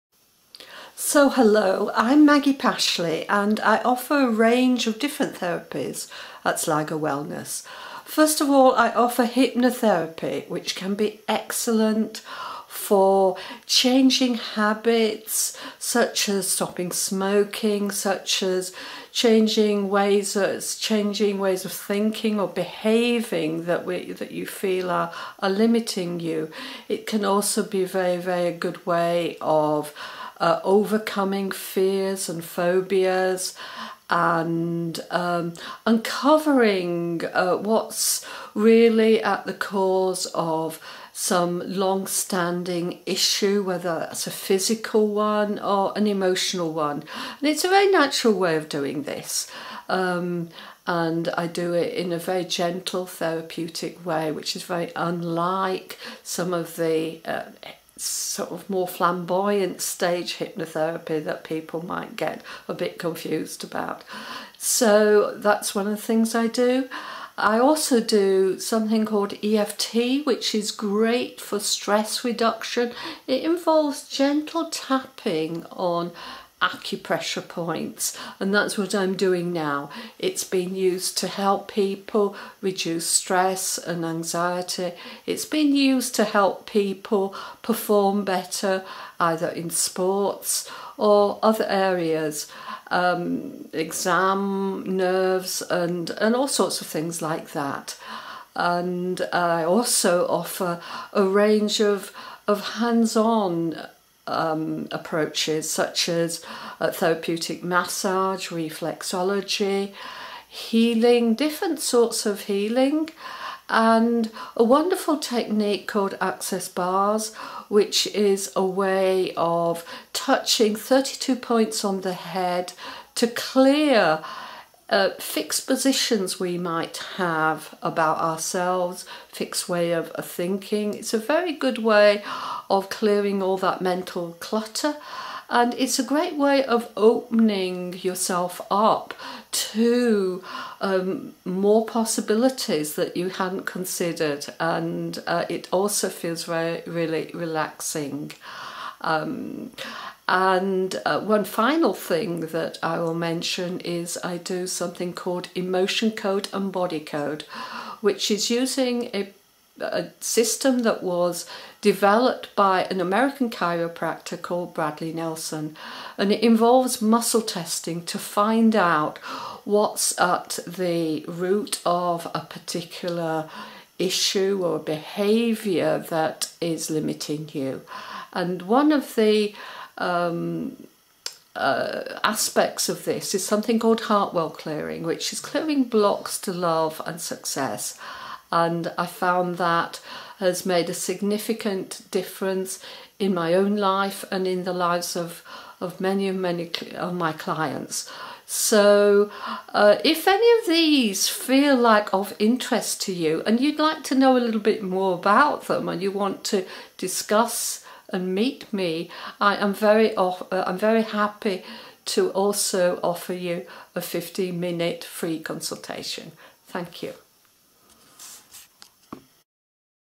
Maggie Pashley introduces her service at Sligo Wellness
Maggie Pashley offers a range of therapies at Sligo Wellness, ranging from hypnotherapy and EFT to help change limiting behaviours, overcome fears and phobias and increase confidence to massage, healing and Access Bars. She also offers free 15 minute consultations. To see the full range of what she offers